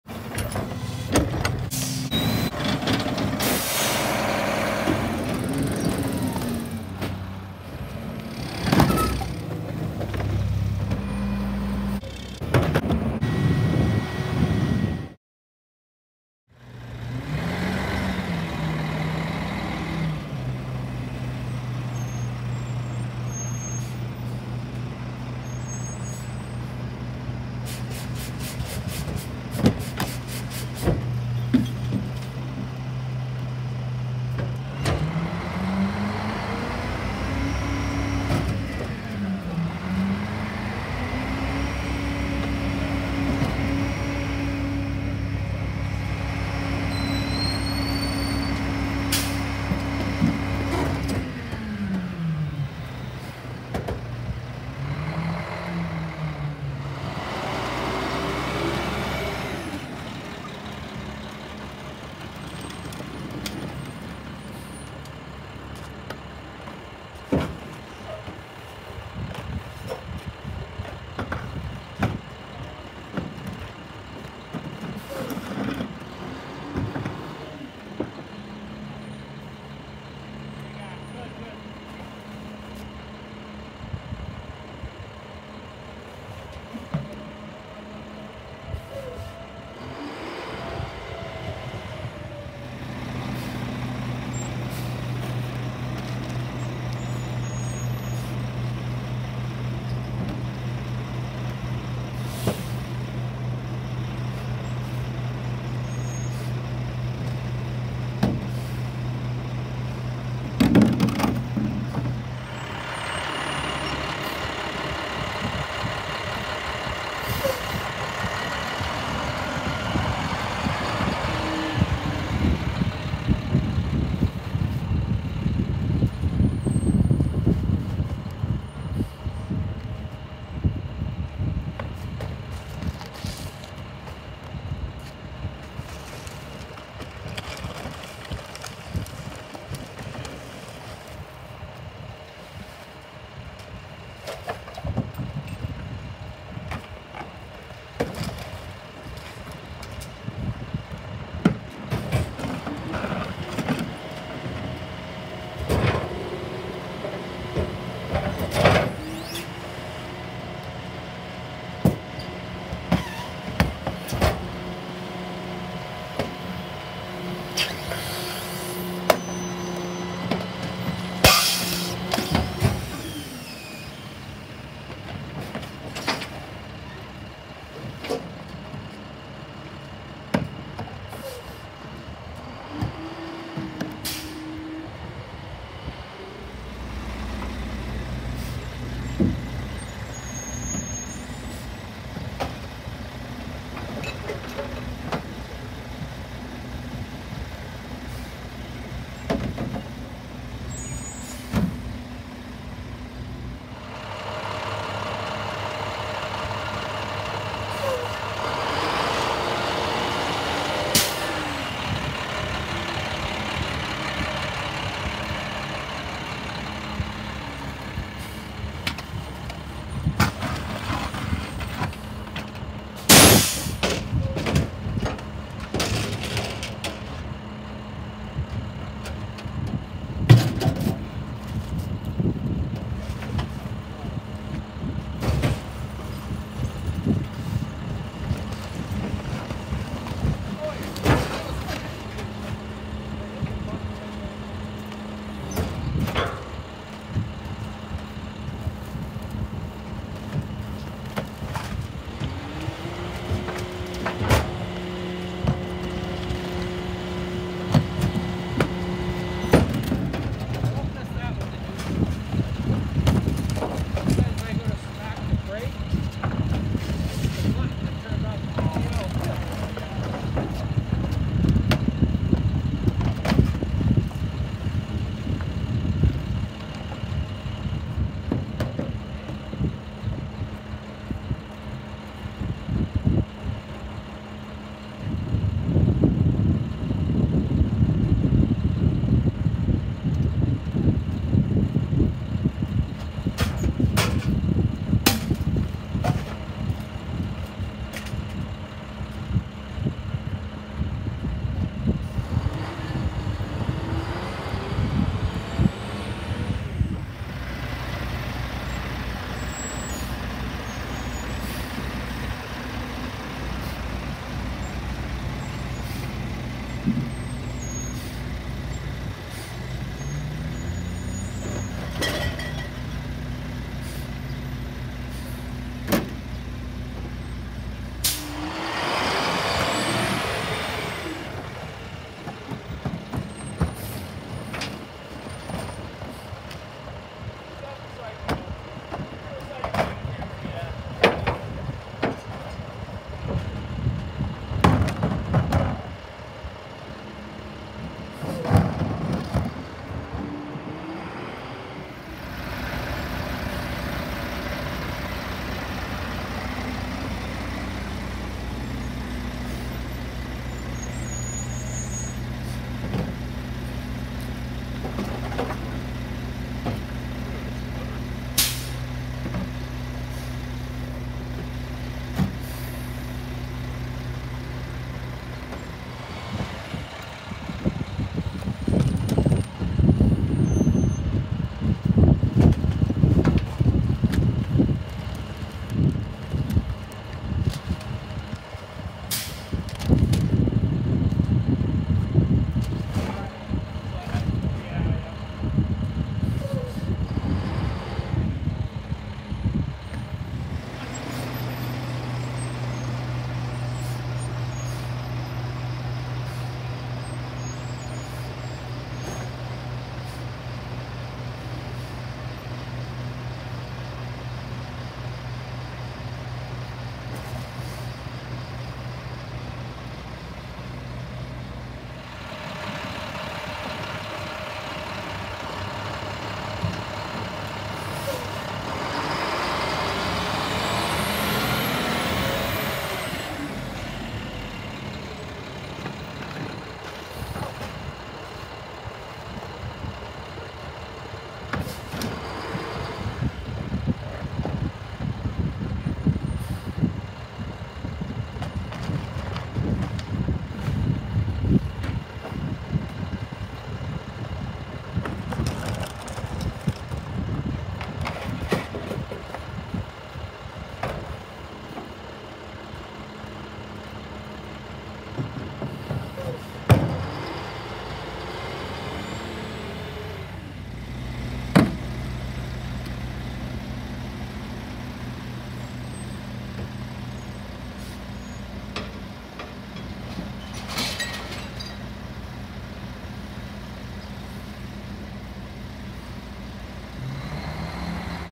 East Rochester Garbage Truck Compilation
Welcome back to East Coast Refuse. Although the Loadmaster is cool the Sterling McNeilus is the star of the show. The Sterling handles all the recycling Monday through Friday in East Rochester. I was planning on getting a full vid of the Sterling but decided to go film their new truck instead. Enjoy - East Coast Refuse

East Rochester DPW This video is for entertainment only not to get a driver or helper in trouble.